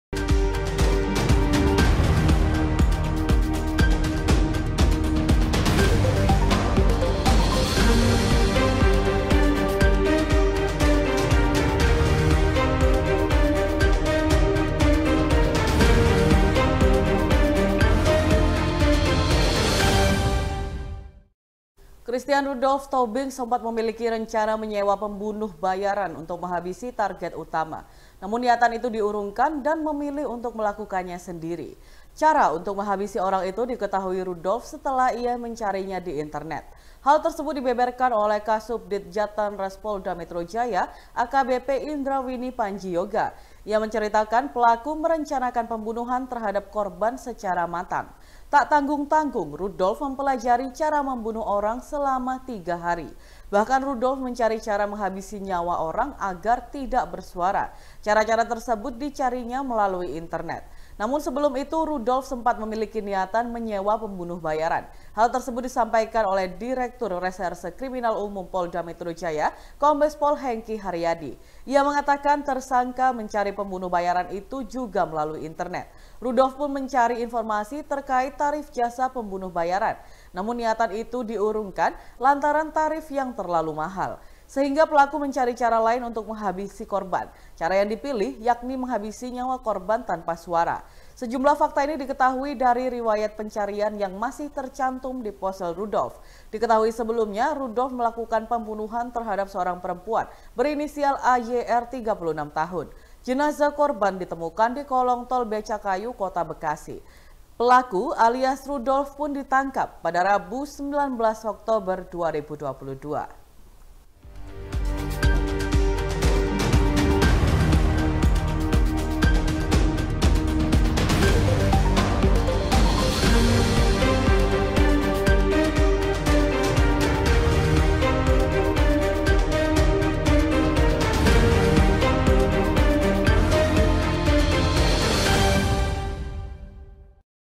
CARI CARA MEMBUNUH TANPA SUARA, Rudolf Ngaku Belajar di Internet
SEKILAS JAWA TIMUR - Fakta-fakta baru kasus Rudolf Pembunuh terus bermunculan.

Kabar terbaru menyebut, Rudolf mengaku awali niat membunuhnya dengan berselancar di internet hingga 3 hari.

Secara spesifik, dia mencari cara terbaik melakukan pembunuhan tanpa suara.

Dia bahkan sampai rela menyewa satu unit apartemen untuk satu hari demi bisa memuluskan niatnya.